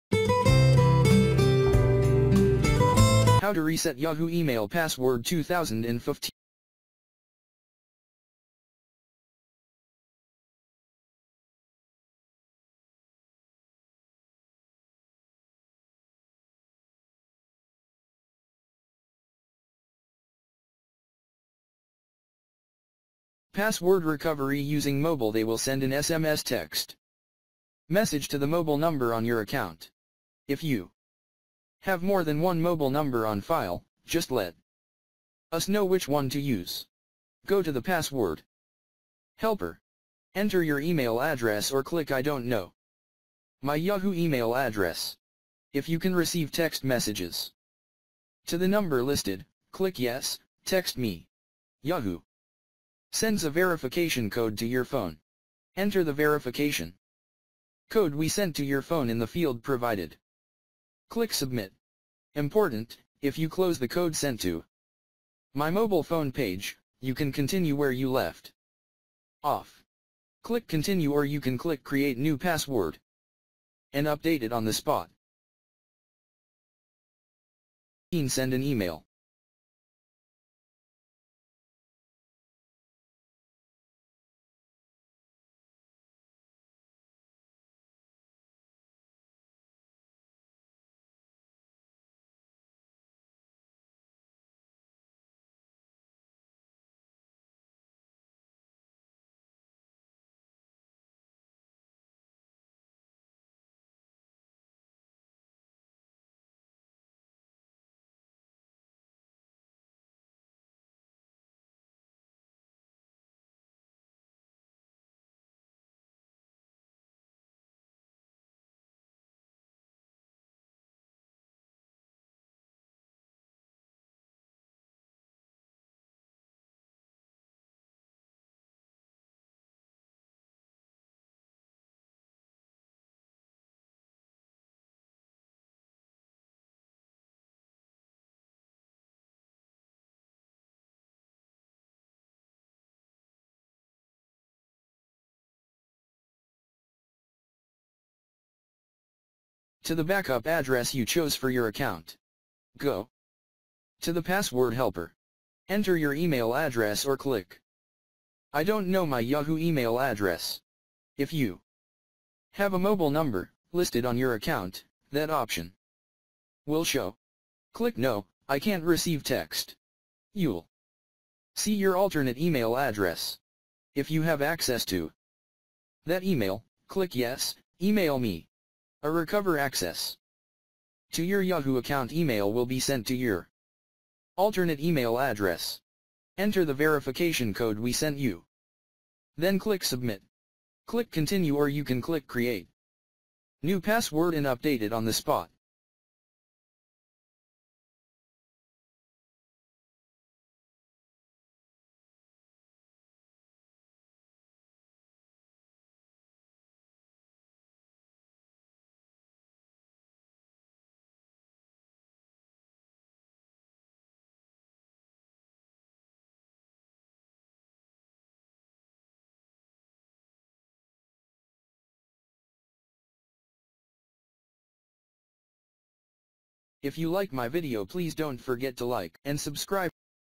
How to Reset Yahoo Email Password 2016 - Yahoo Email Services
If you forgot the password for your Yahoo account but don't have access to a computer, you can reset your password using a mobile device.
easy to get back in.

Resetting password using Mobile
Go to the Password Helper.
If you can receive text messages to the number listed, click Yes, text me.
- Yahoo sends a verification code to your phone.
Enter the verification code we sent to your phone in the field provided.
Click Submit.
Important: If you close the "Code sent to my mobile phone" page, you can continue where you left off.
Click Continue or you can click Create new password and update it on the spot.

They will send an email to the backup address you chose for your account.
- If you have a mobile number, listed on your account, that option will show.
Click No, I can't receive text.
Enter the verification code we sent you, then click Submit.